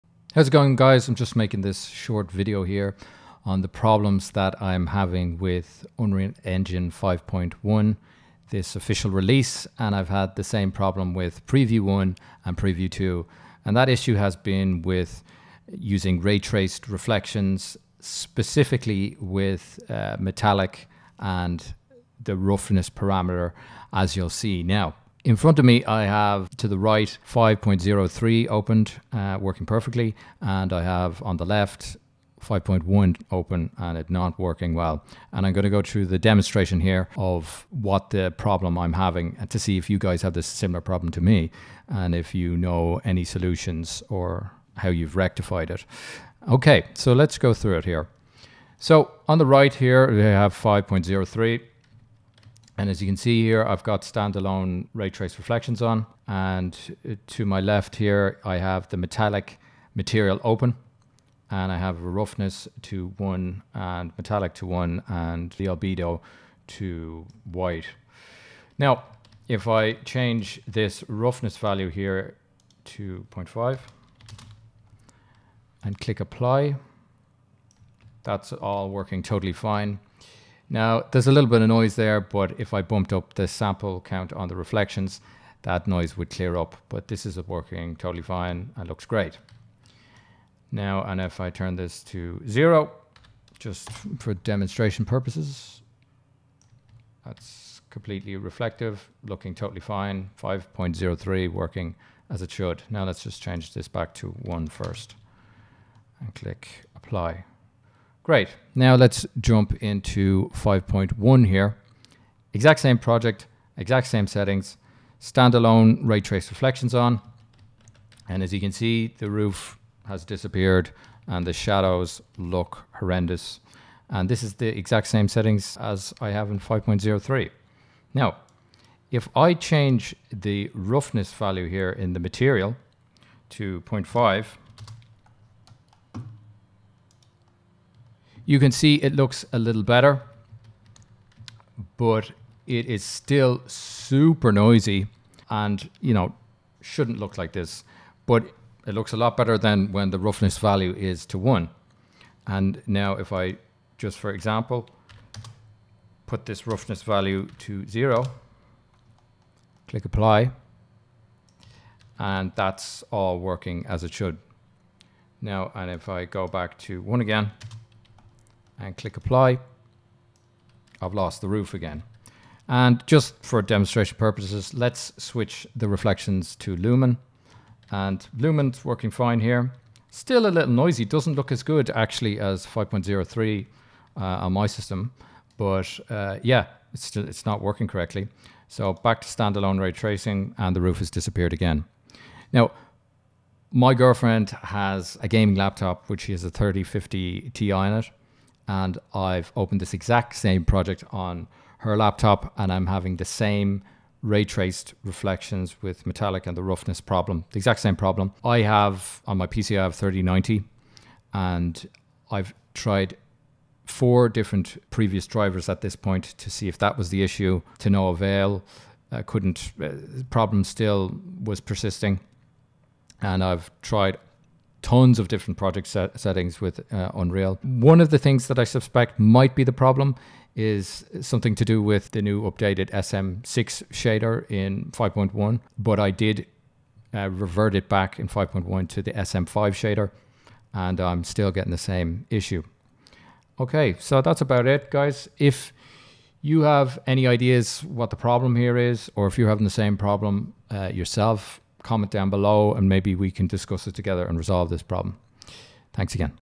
Unreal Engine 5.1 Ray Traced Reflections Issue/Bug Metallic Black And Noisy
3090
5950x
64GB Ram DDr4

Any Ideas Let me know? I'm pulling my hair out here!